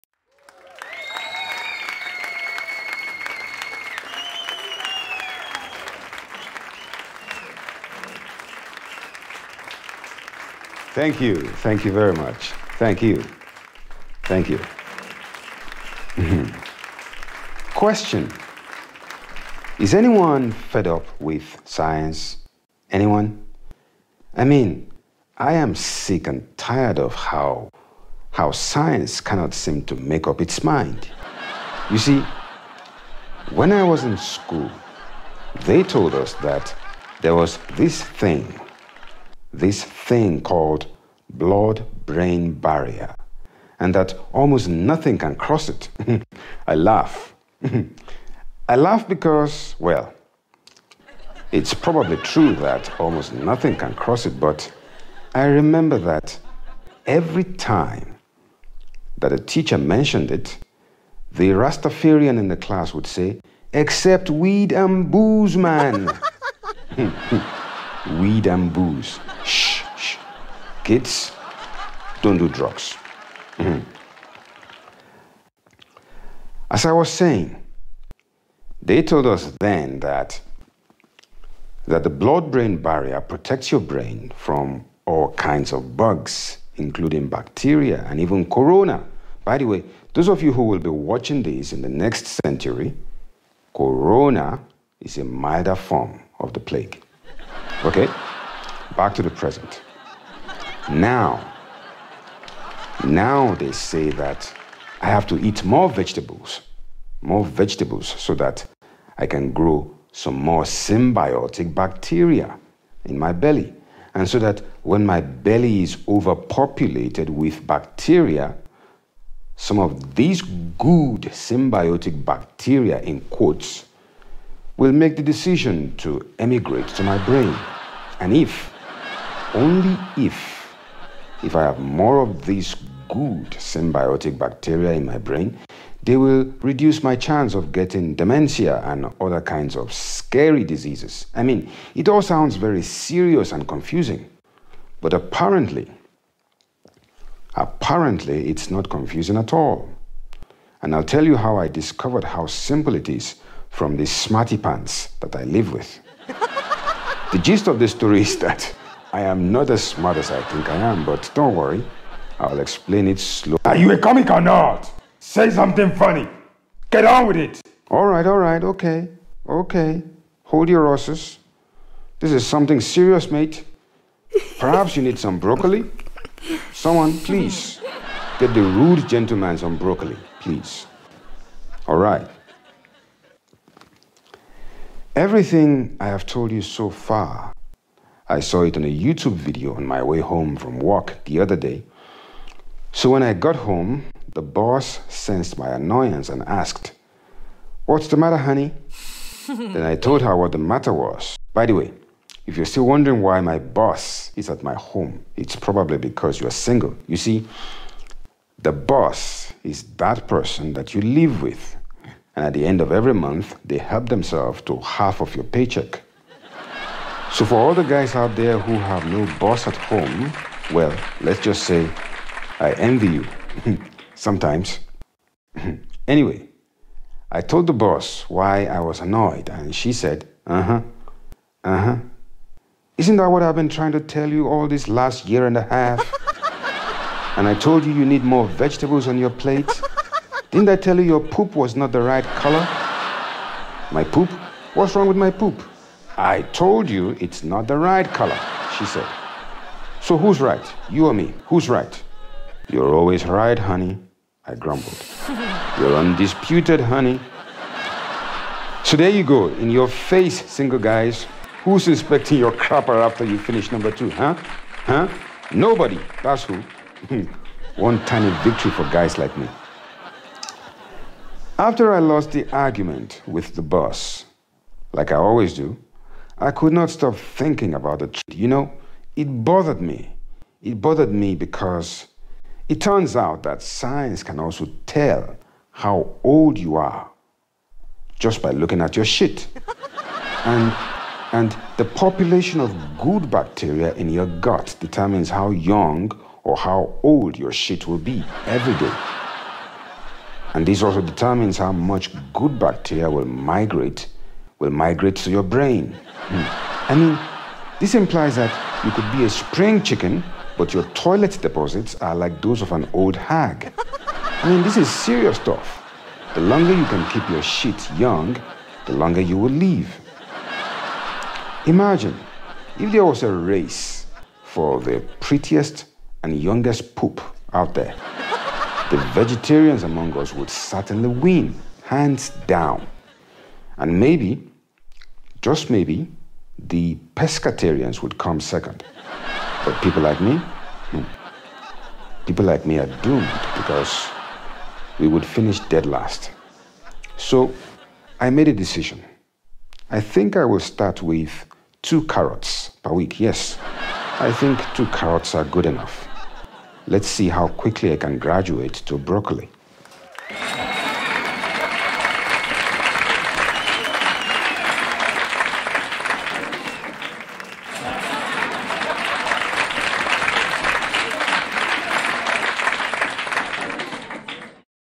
The secrete of forever young
BMBH episode 2.
This is no laughing matter.  Stay young by keeping your poop young.  Keep your poop young, by eating lots of vegetables to grow friendly gut microbiota.  Some of these gut micro-organisms will find their way to your brain to keep your brain healthy too.  Then you will be truly hale in mind and body.

Free footage from pexels:
Video by Nataliya Vaitkevich from Pexels